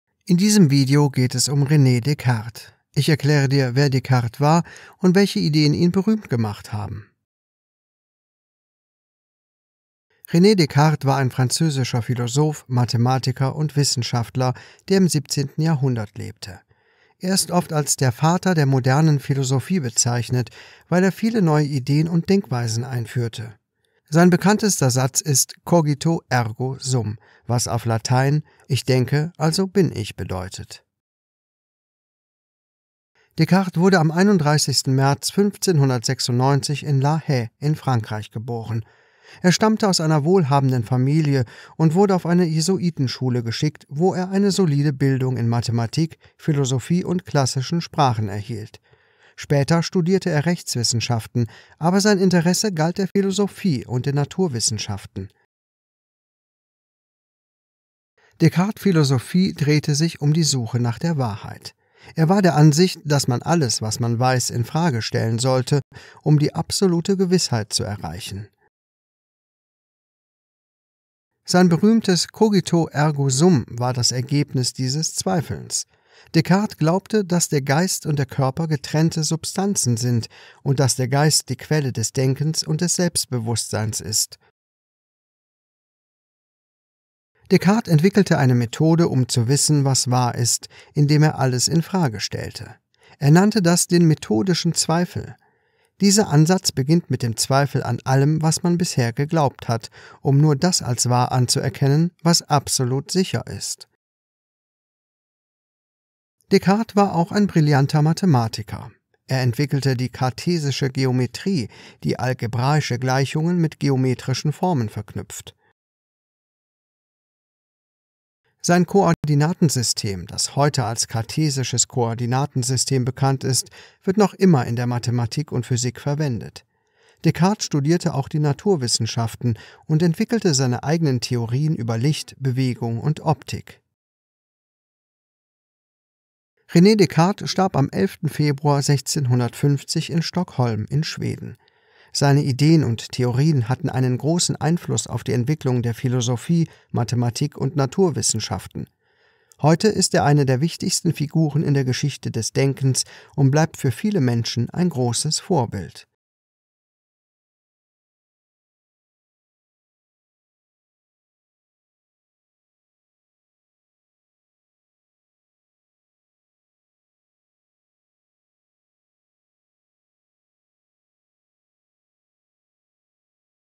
René Descartes
Erfahre in weniger als 3 Minuten, was du über René Descartes wissen solltest!